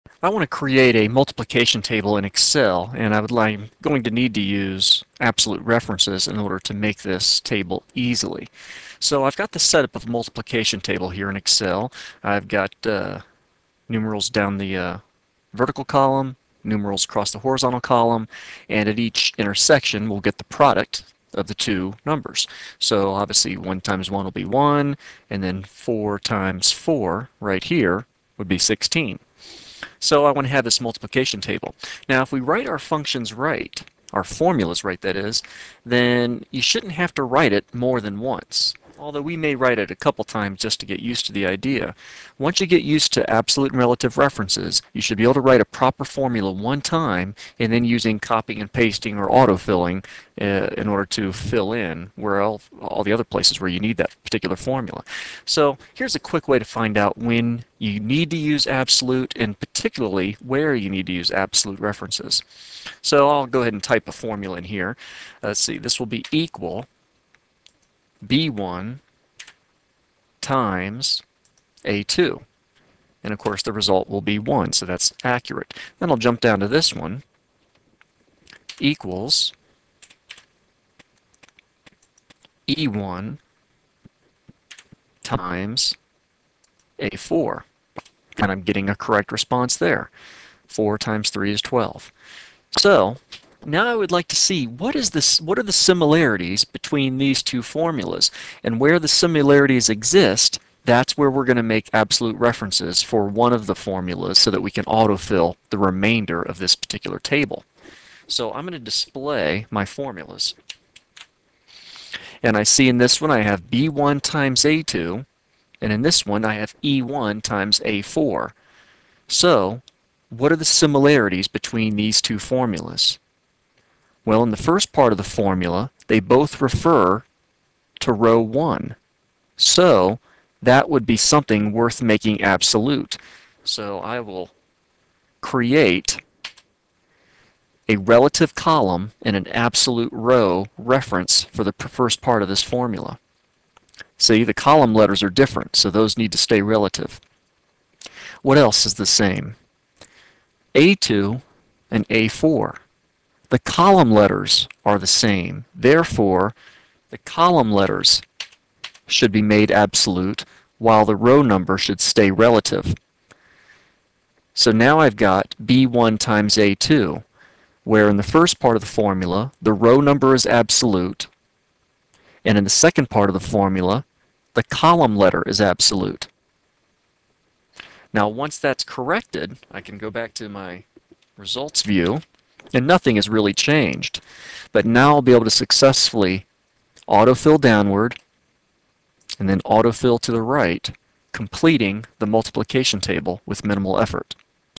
When Are Absolute References Helpful in Excel?
making a multiplication table in Excel as an exercise to help determine what portions of a cell reference should be made absolute prior to copying the formula.